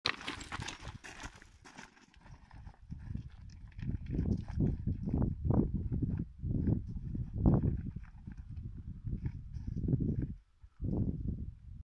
Walking mountains 2, Precise rapid feet
Quick scanning and interaction with precarious surface is the fun of it.
The runner has to place the feet with laser precision in very short amount of time.
P.S. for further explanation see Walking mountains 1